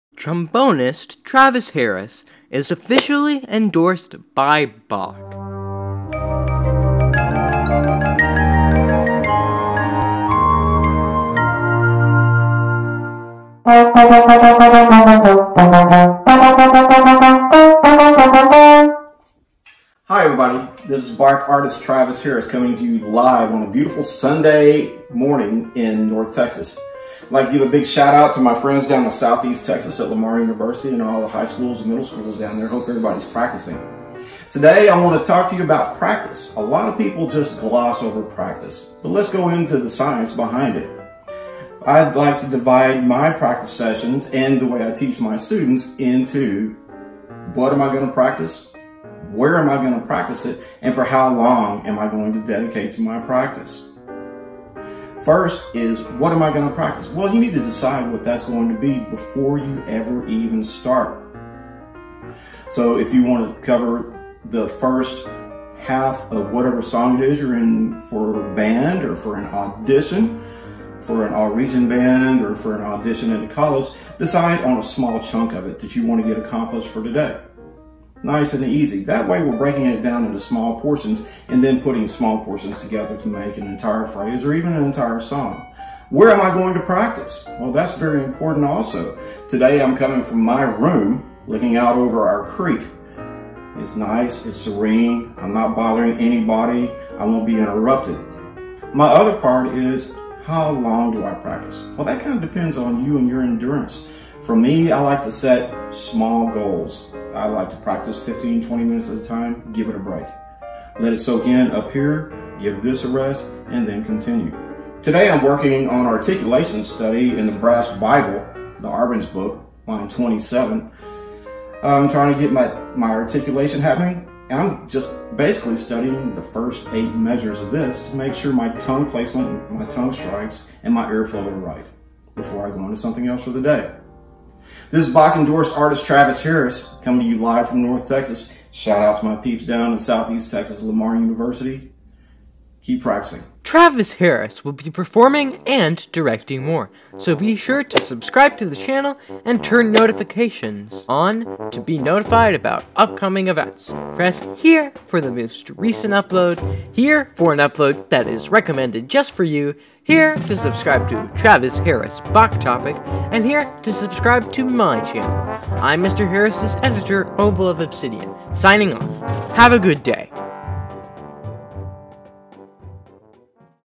Travis Harris – Practice Habits
Endorsed Bach trombonist Travis Harris talks about practice habits.